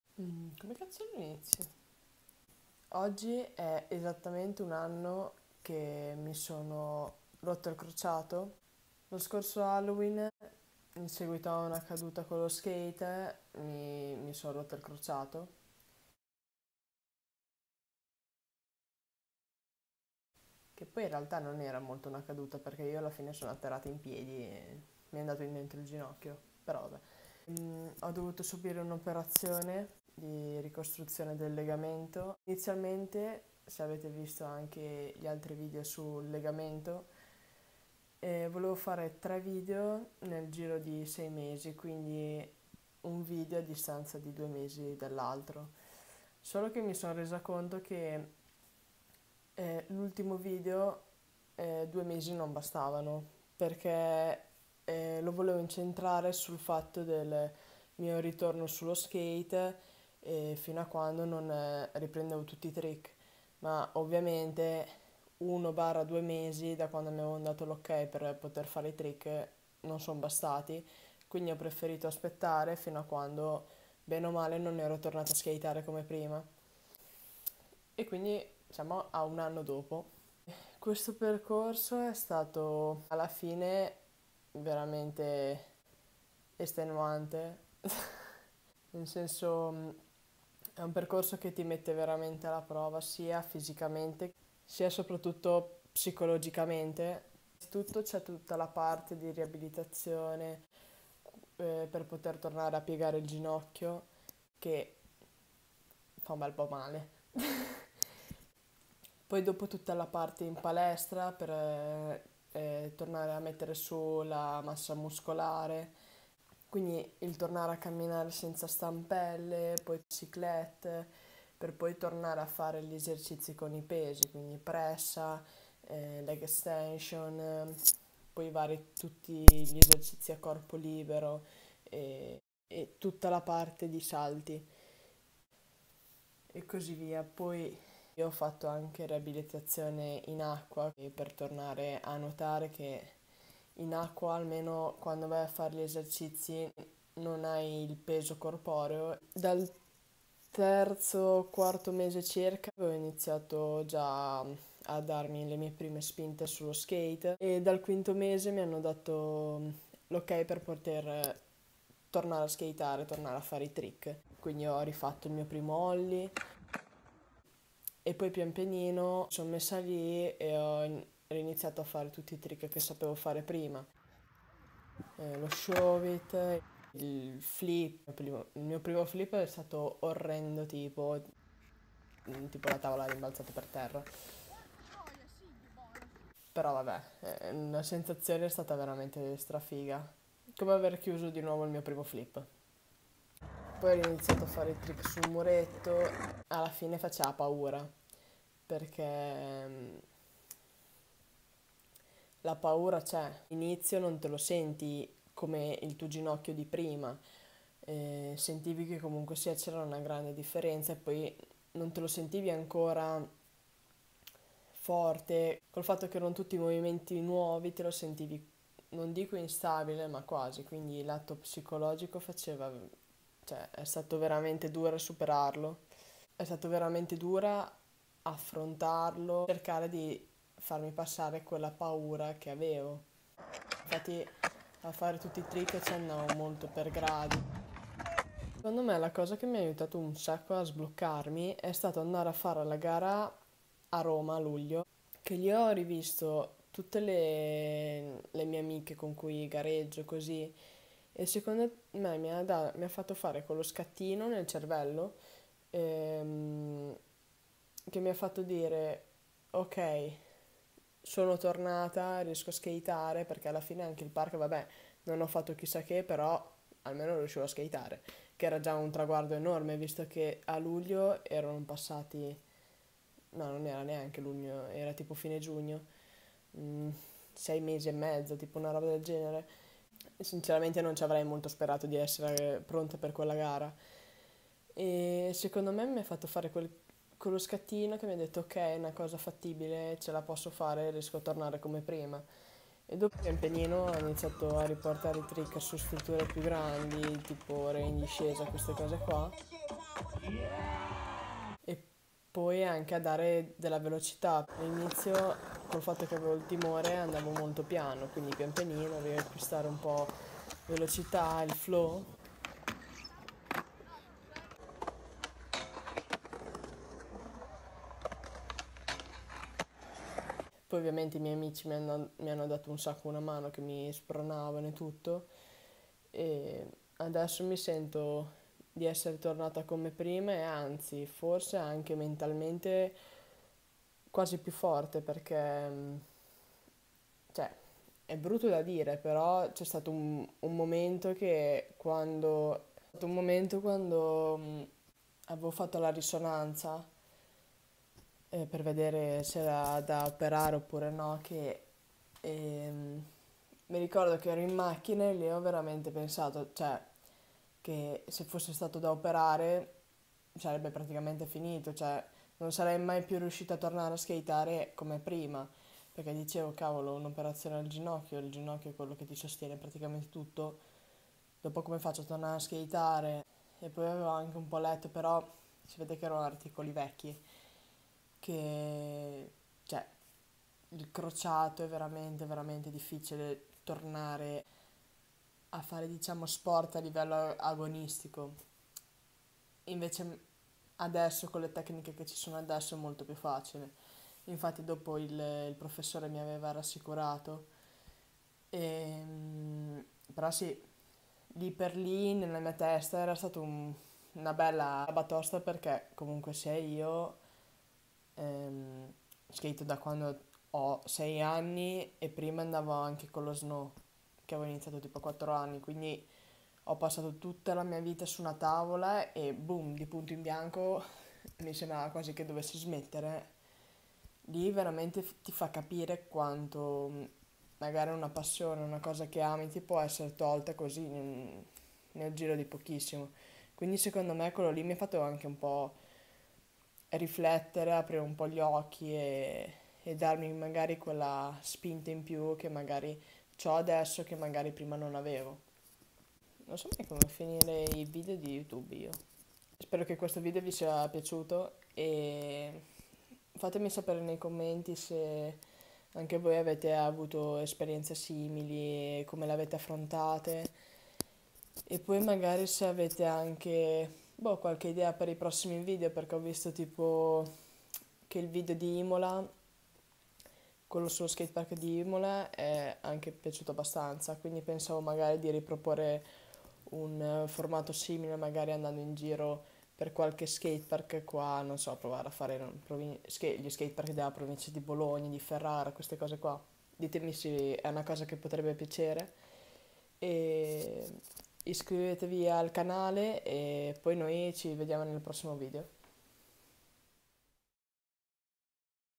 MI SONO ROTTA IL CROCIATO PT.3 - Un anno dopo (VlogIT)
Esattamente un’anno fa mi sono rotta il crociato. Questo sarà l’ultimo video di questa “serie” che ho iniziato quando mi sono fatta male e dove spiego il mio percorso per tornare sullo skate.

Un grazie a @italianskateboarding @fisr @giovannidemartinofkt @european_medical_center per avermi seguito durante questo lungo percorso

👉🏻LINK